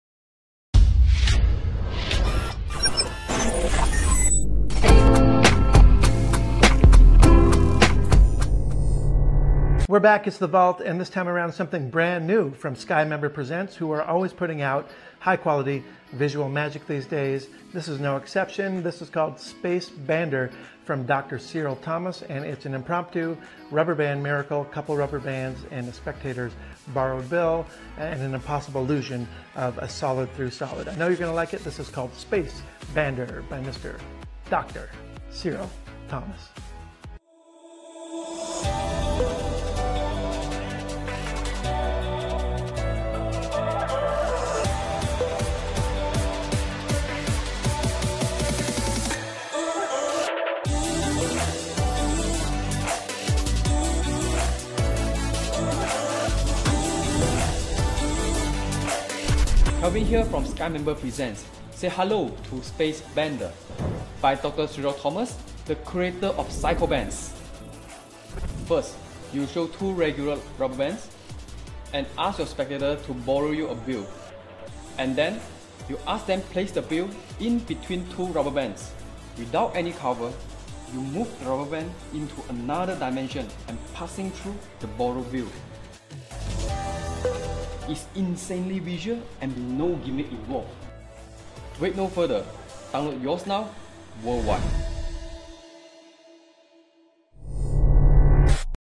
The Vault - Skymember Presents Space Bander by Dr. Cyril Thomas - Madanai Magic
The creator of Psychobands, Dr. Cyril Thomas is back with another piece of eye candy!

Say hello to Space Bander; an extremely visual rubber band penetration effect that is suitable for everyone. It is practical, easy to do, and yes, it is insanely visual.

You present two rubber bands and stretch them out. You borrow a bill from your spectator and ask them to place it in between the two rubber bands. Without any cover, you magically move the rubber bands into another dimension and they penetrate the borrowed bill. The borrowed object is not limited to the bill; it could be playing cards, credit cards or even their finger.

In this project, Calvin Liew, Creator of the Art of Rubber Band (AORB) will teach you all of the handlings that you need to know to perform this in real life. Detailed instructions are in English.

Wait no further, download Space Bander now and fool your friends and family during this quarantine period.